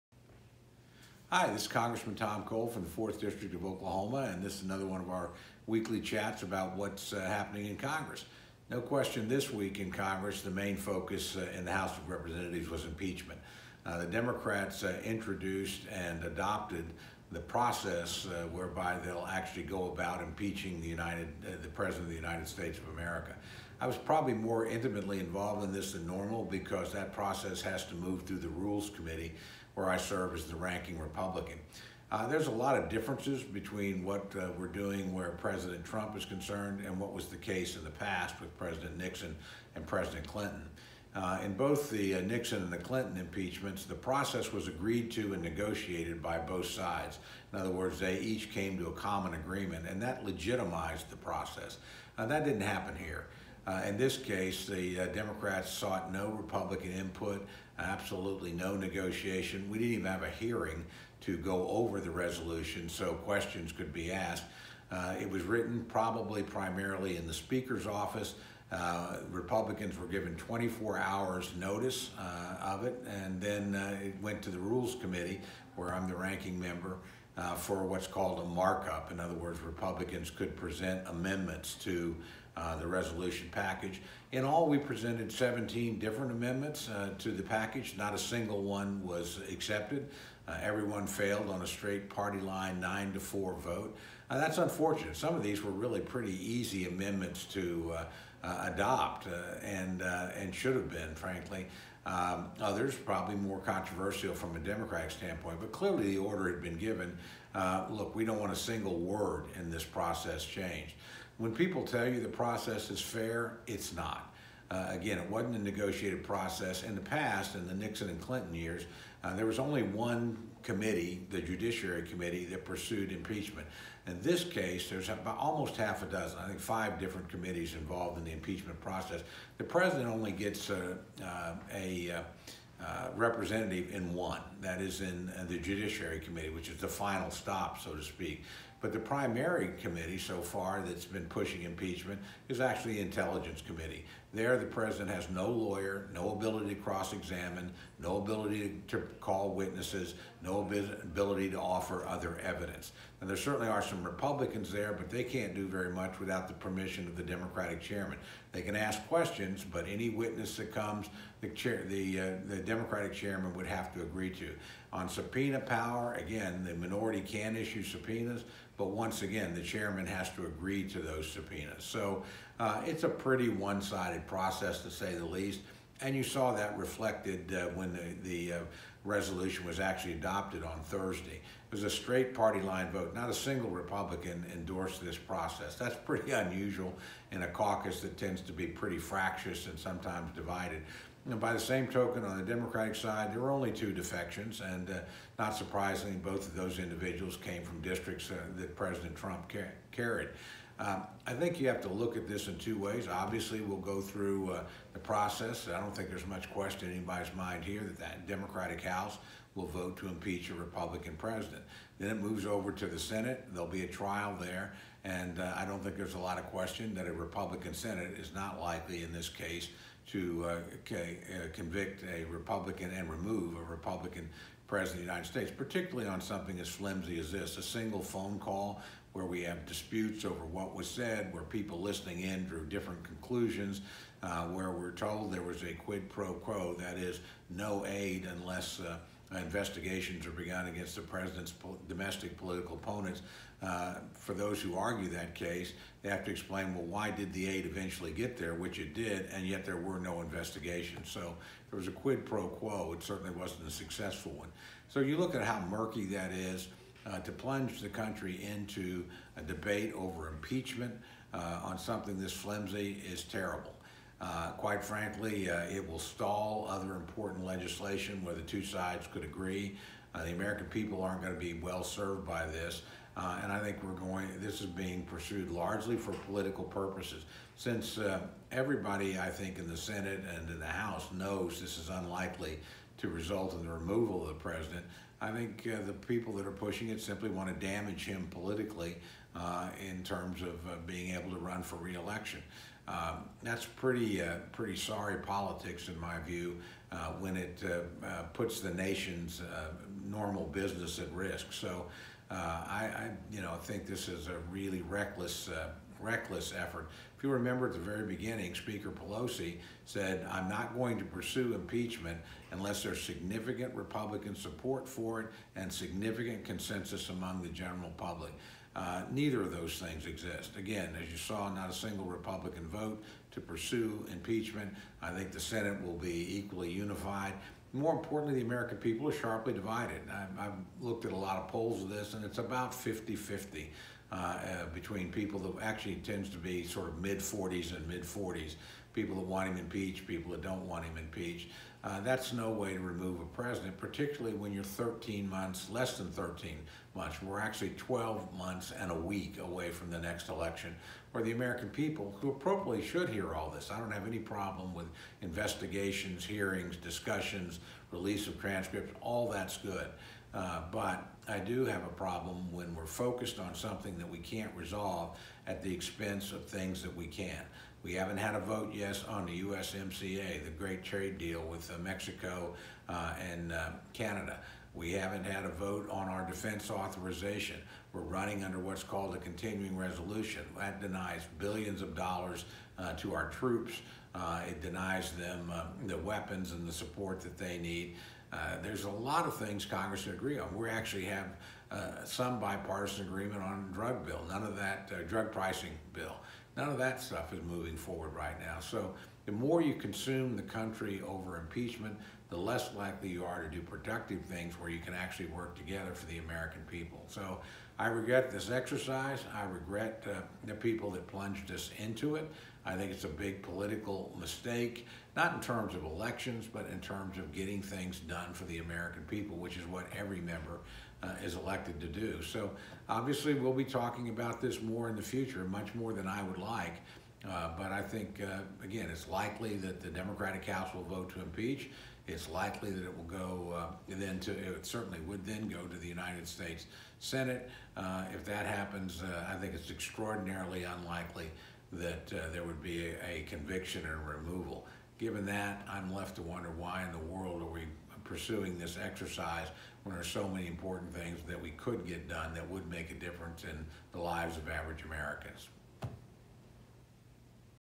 Rep. Cole's Weekly Chat (11/1/19)
Rep. Tom Cole (OK-04) discussed the impeachment process resolution that was written solely by Democrats, marked up in the House Rules Committee and passed only with Democratic support in the whole House. The top Republican on the Rules Committee, Cole explained why the process set forth in the resolution is not fair, open or transparent. (November 1, 2019)